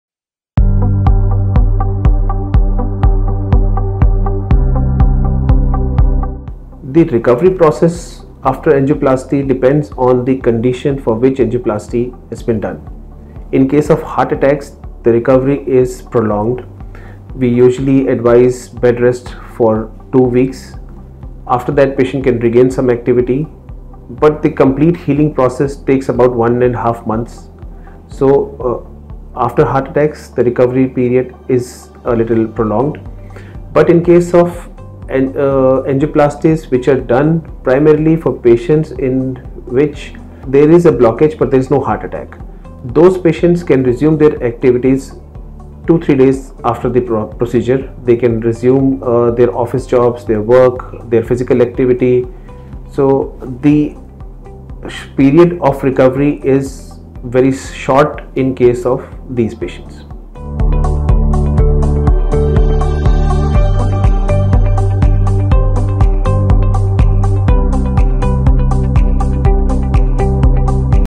Recovery Process of Angioplasty | Manipal Hospital Gurugram | Dr Dixit Garg
Understanding the Recovery Process of Angioplasty: A Message from Dr. Dixit Garg. Dr. Dixit Garg discusses the process of recovery after an angioplasty surgery.

Watch the video to learn more about the recovery process after an angioplasty.